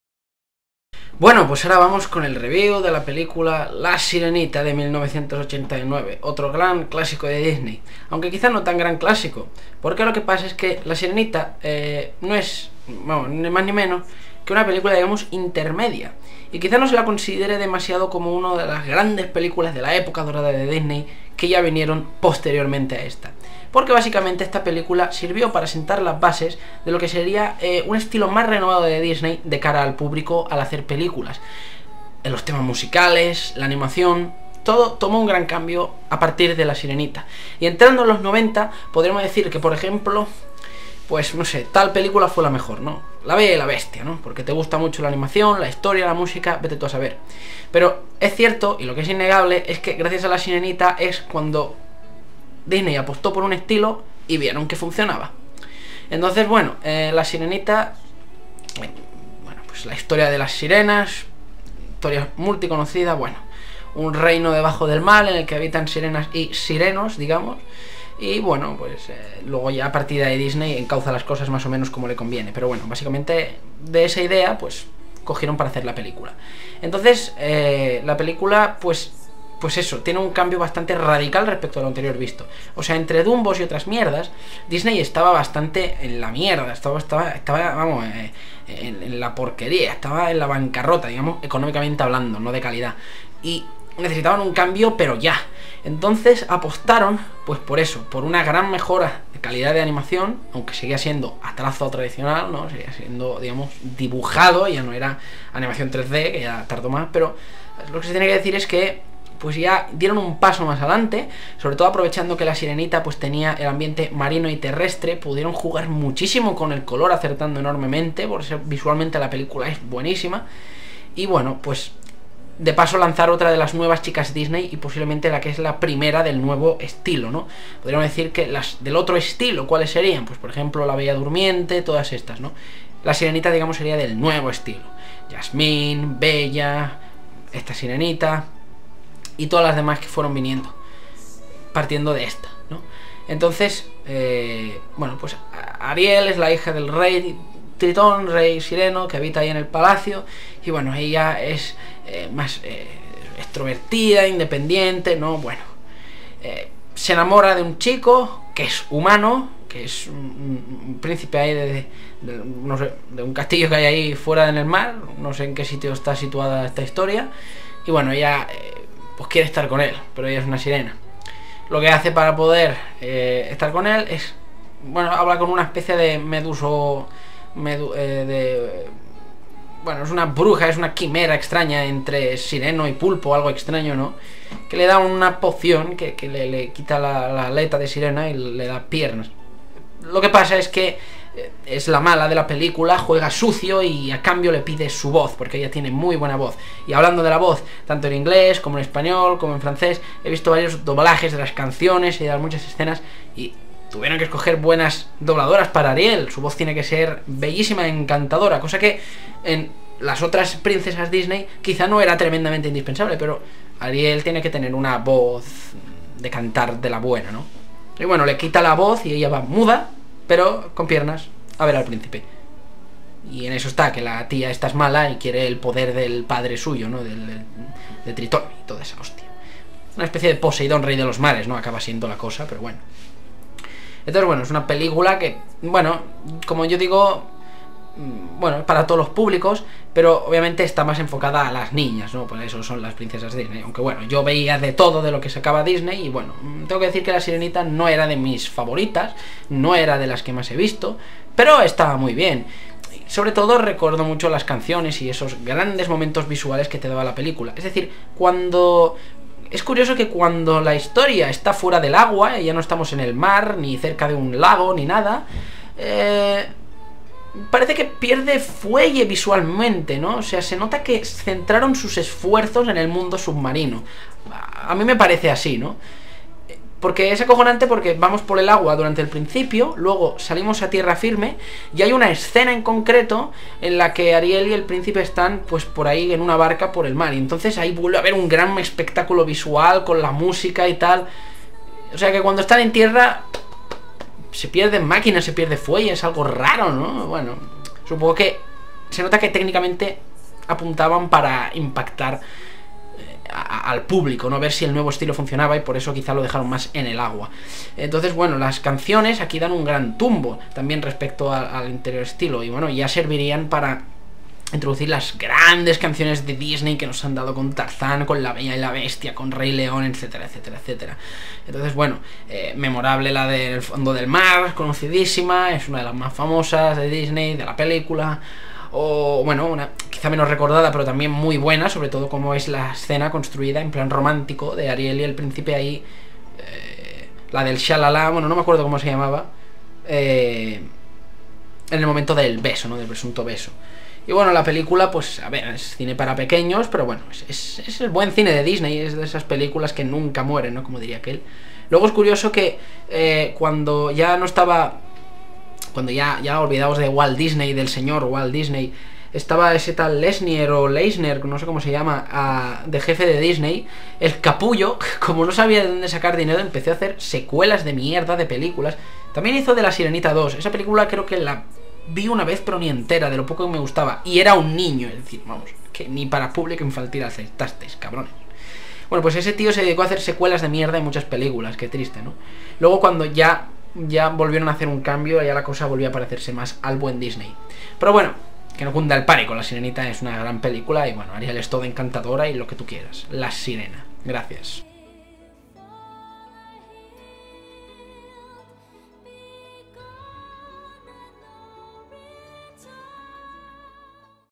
Review/Crítica "La sirenita" (1989)
Review de otra clásico de la animación basado en al historia de Hans Christian Andersen, La Sirenita, que dió paso al nuevo y espectacular estilo más recordado de la casa Disney... . ¡Deja tu 👍🏻 y dale a la 🔔 para que YouTube te notifique SIEMPRE que suba contenido!

🛠Diseño del canal y RRSS realizado por Pep Filmmaker🛠